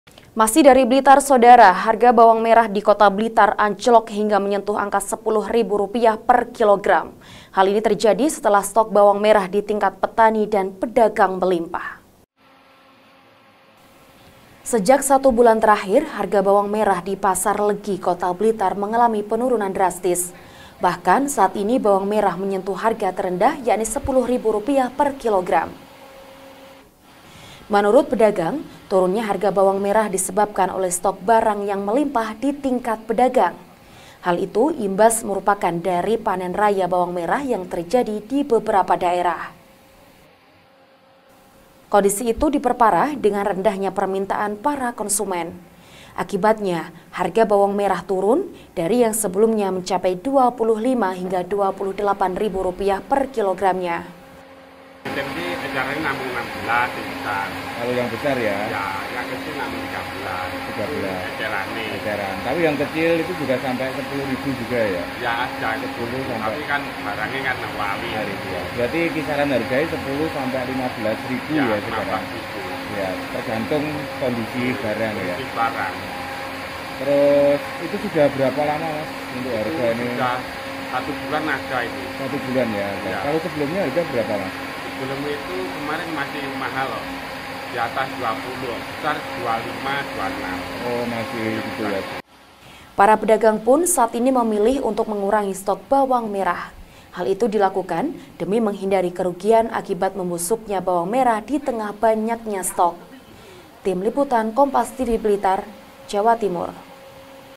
Stok Melimpah, Harga Bawang Merah Anjlok Hingga Rp 10.000 per Kilogram
BLITAR, KOMPAS.TV - Sejak satu bulan terakhir, harga bawang merah di pasar Legi, kota Blitar, mengalami penurunan drastis. Bahkan saat ini bawang merah menyentuh harga terendah, yakni 10 ribu Rupiah per kilogram.

Menurut pedagang, turunnya harga bawang merah disebabkan oleh stok barang yang melimpah di tingkat pedagang. Hal itu merupakan imbas dari panen raya bawang merah, yang terjadi di beberapa daerah.

Kondisi itu diperparah dengan rendahnya permintaan para konsumen. Akibatnya, harga bawang merah turun, dari yang sebelumnya mencapai 25 hingga 28 ribu rupiah per kilogramnya.

Para pedagang pun saat ini memilih untuk mengurangi stok bawang merah. Hal itu dilakukan demi menghindari kerugian, akibat membusuknya bawang merah di tengah banyaknya stok.